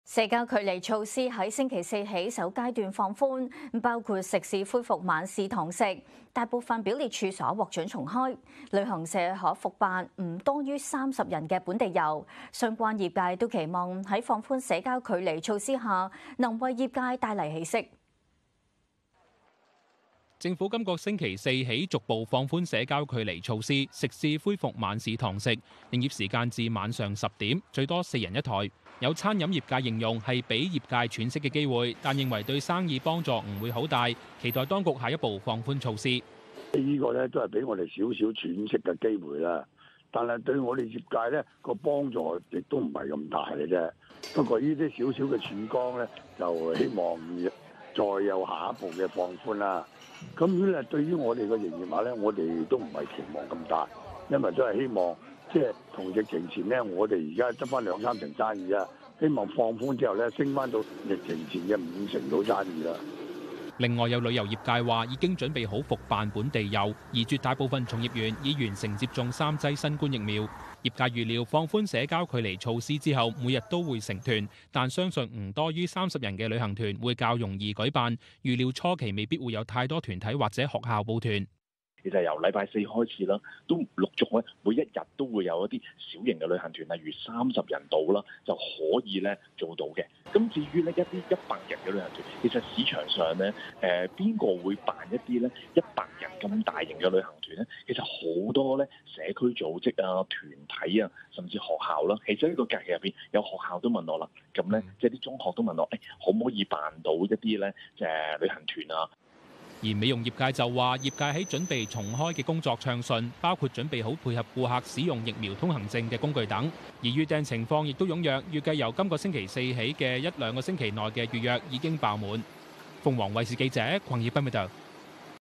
香港疫情｜本周四放寬防疫措施：飲食業界指對生意幫助不會很大【香港V】
政府本周四起逐步放寬社交距離措施，食肆恢復晚市堂食，營業時間至晚上10時，最多4人一枱。有餐飲業界形容是給予業界喘息機會，但認為對生意幫助不會很大，期望當局下一步放寬措施。